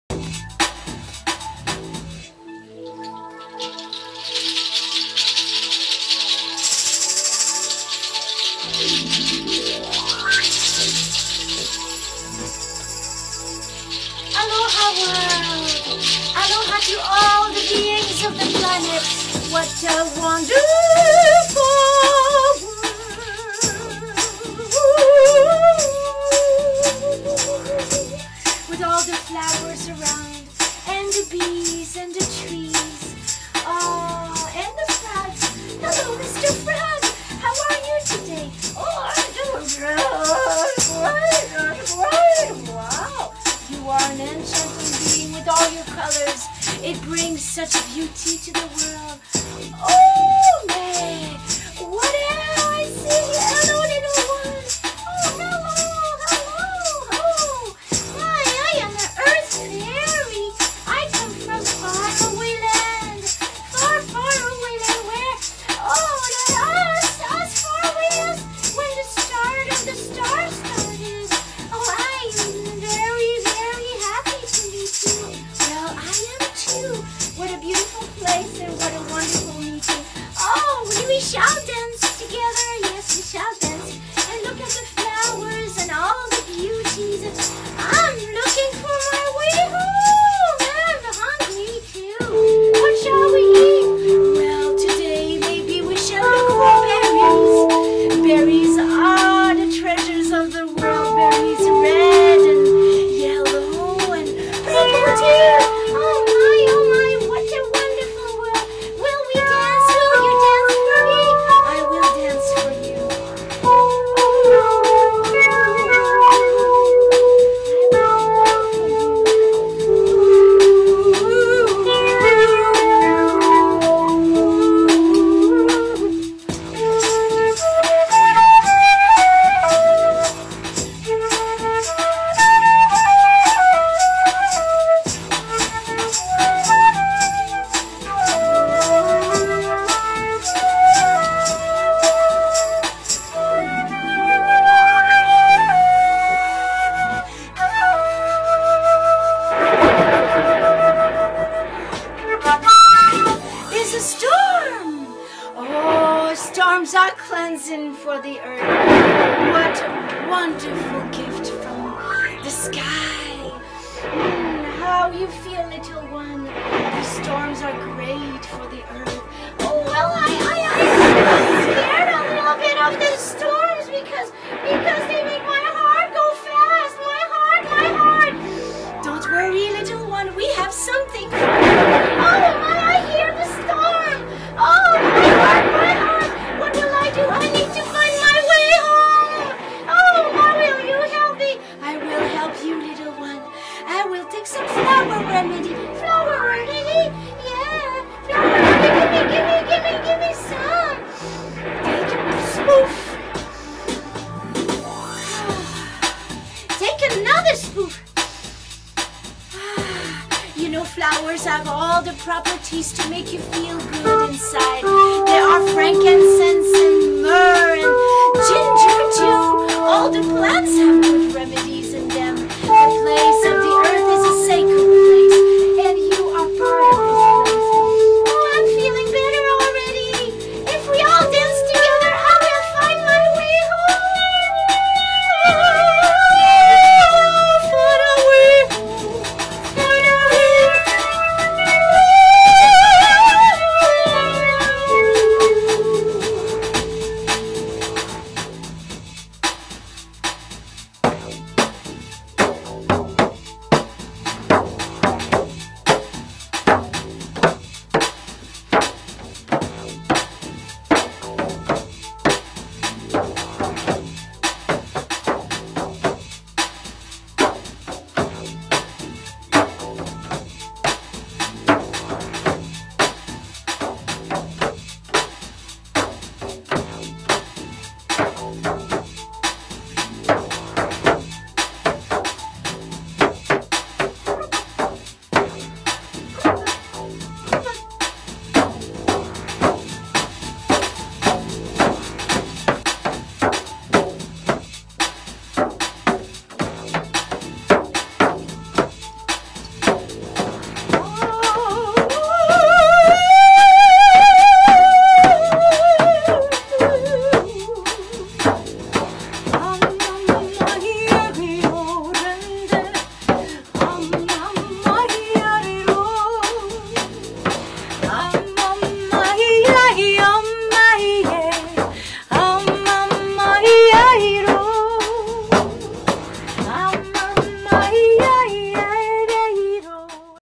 The Magic Fairy Garden
Just a  sketch of a fairy Tale from Starchild  entertainment ...
A fairy  is lost and is looking for her way home..The Magic Garden...